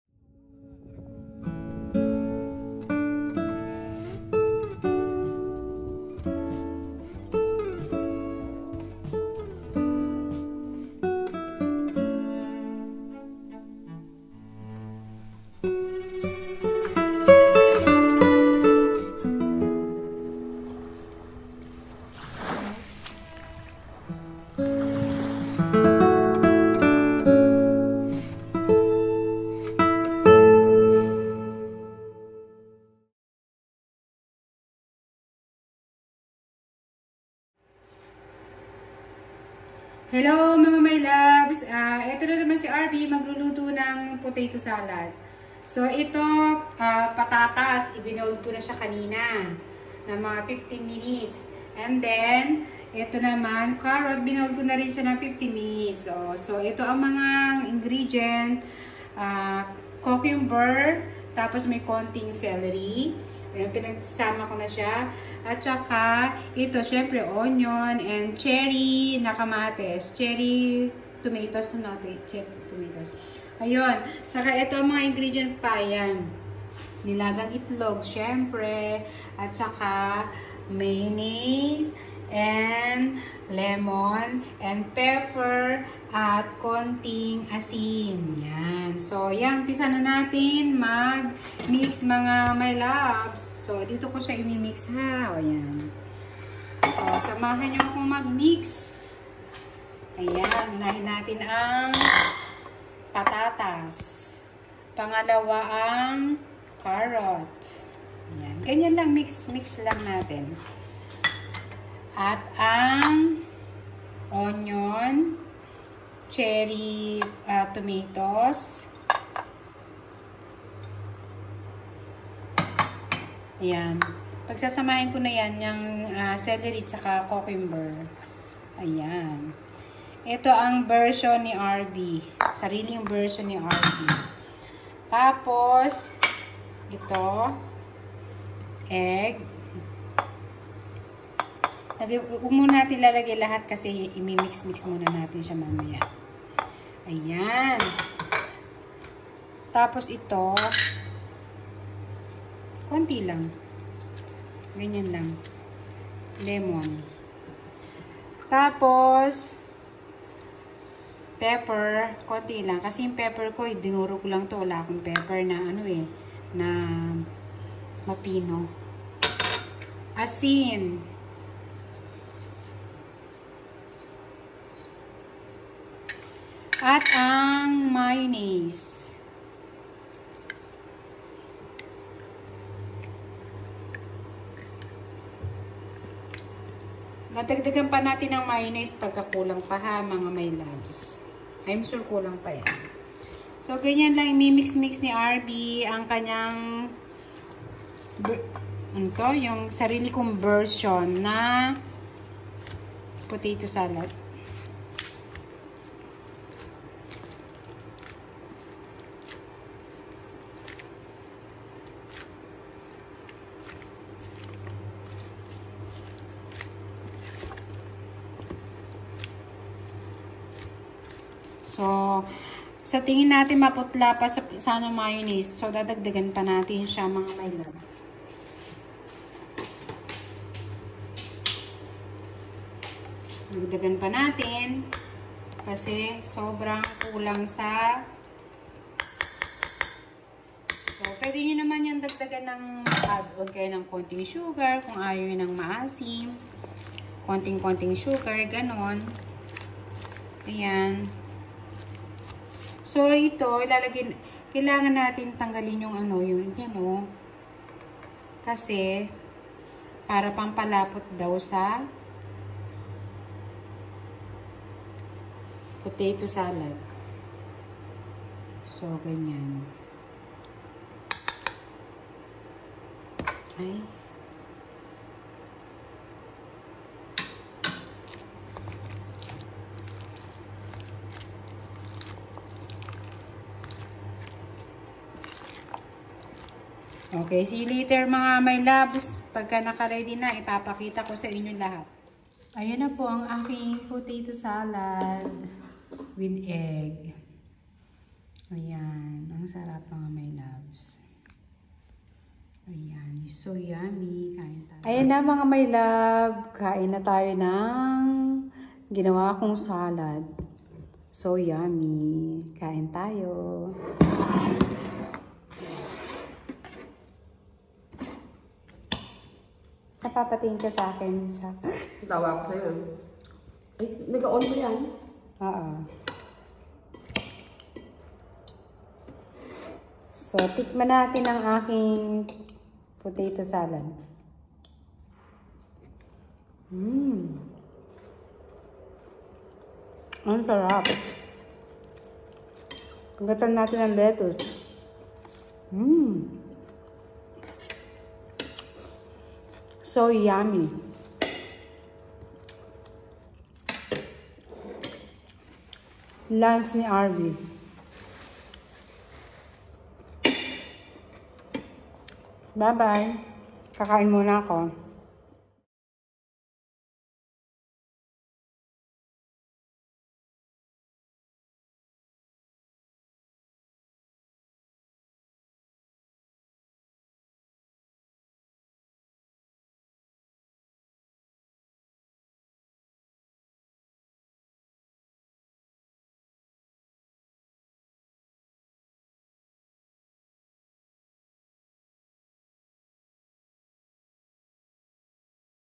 My Own Version Potato Salad healthy food# veges# yummy#
Fresh na fresh at healthy foodies # satisfying  # my own recipe # carrot# cheery tomato# onion # cocomber#letuce#🥗🥗🥗